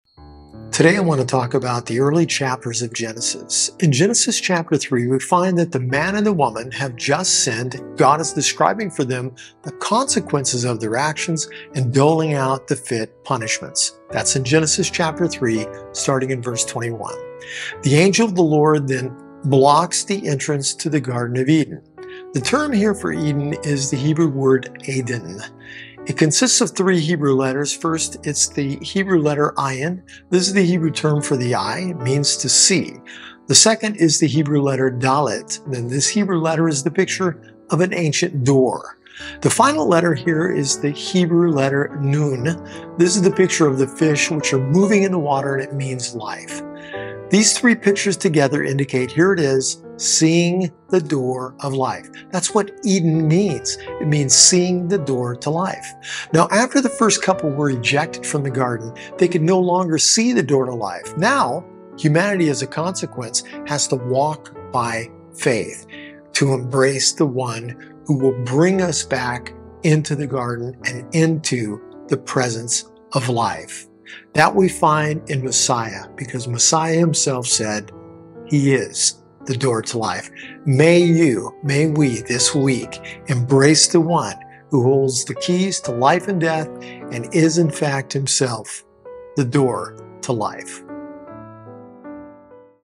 Tidbits from the Bible S01E04 The Door to Life
Join Rik as he discusses the Messiah and the Paleo-Hebrew word for "Eden."